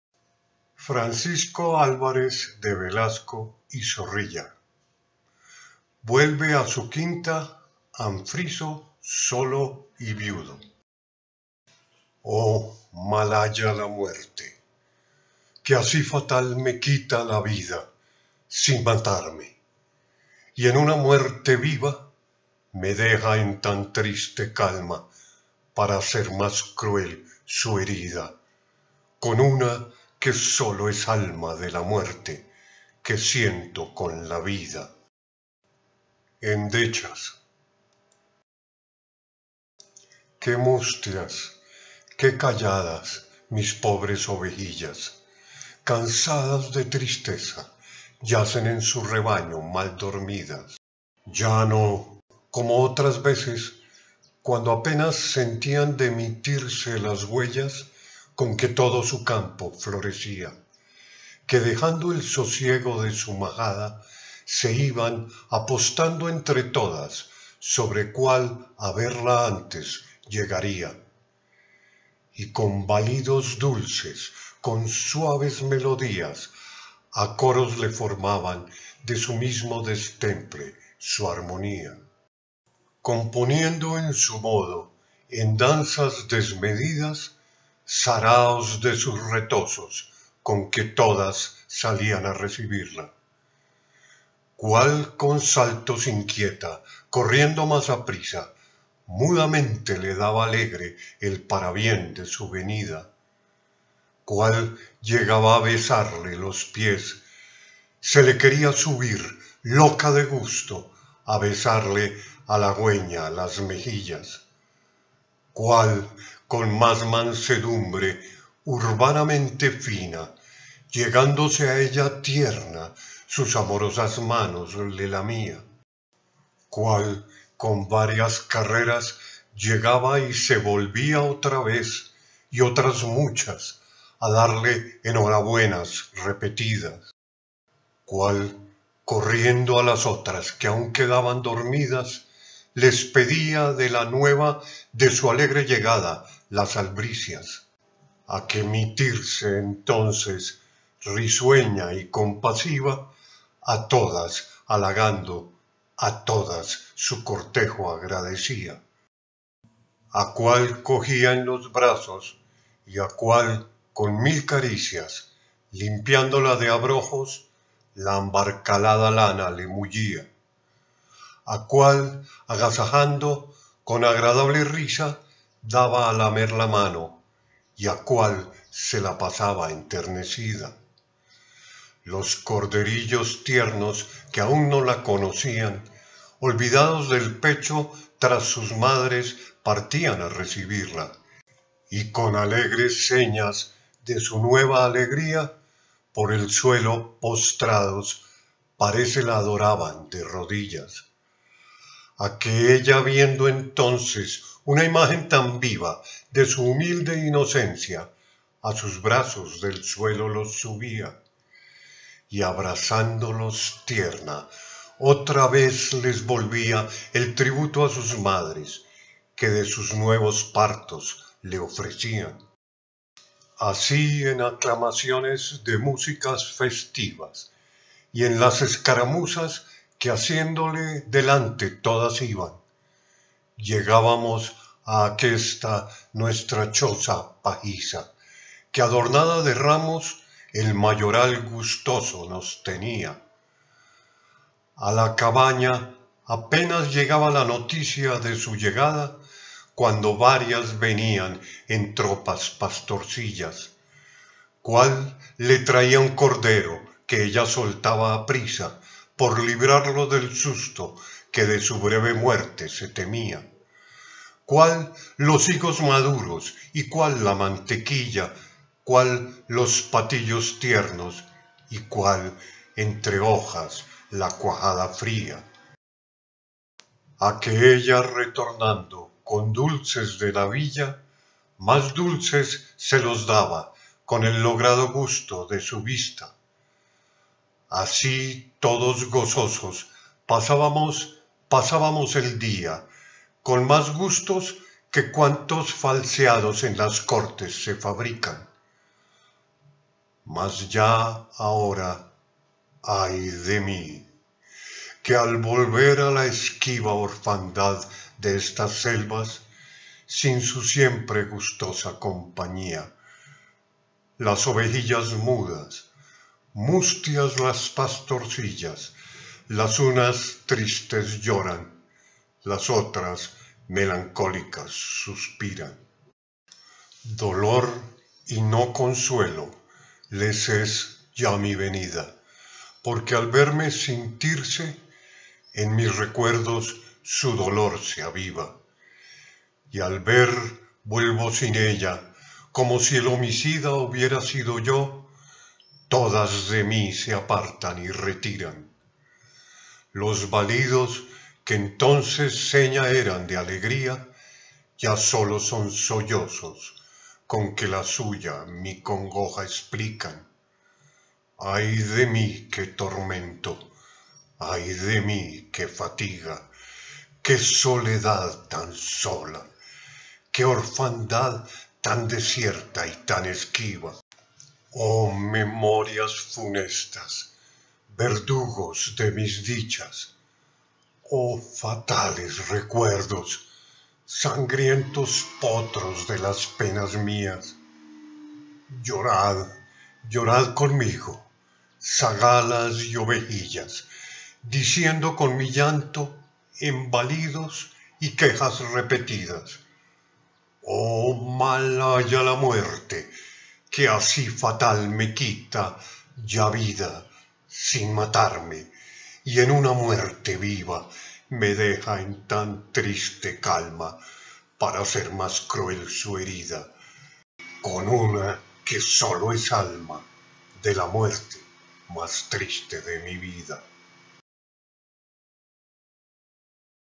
"Vuelve a su quinta, anfriso, solo y viudo" Francisco Álvarez de Velasco y Zorrilla
Francisco Álvarez de Velasco y Zorrilla (Santa Fe de Bogotá, 1647- Madrid, 1708) cursó sus estudios escolares en el Colegio de San Bartolomé de donde se graduó con apenas 16 años de edad, y donde recibió una rigurosa formación religiosa. A los 16 años también contrajo nupcias con la distinguida señora de sociedad, Doña Teresa de Pastrana y Cabrera. Por muerte del titular, el 17 de diciembre de 1667 fue nombrado gobernador y capitán general de la ciudad y Provincia del Valle de la Concepción de Neiva.

A la triste muerte de Doña Teresa en 1694 se debe la inspiración de: Vuelve a su Quinta Anfriso solo y viudo, poema elegíaco de hondo romanticismo, y del texto Los Novísimos. De esta misma época datan sus poesías morales y las encomiásticas, que fueron producto de sus mejores años en Santa Fe de Bogotá cuando fuera su alcalde. Entre 1690 y 1692 se encuentra con la voz poética de Sor Juana Inés de la Cruz y dedica el resto de sus días al estudio minucioso de la obra de la monja y a su invocación como mujer. En 1703 se publicó la “Rhythmica Sacra, Moral y Laudatoria”. Es posible afirmar que la obra de Álvarez, es precursora del neoclasicismo.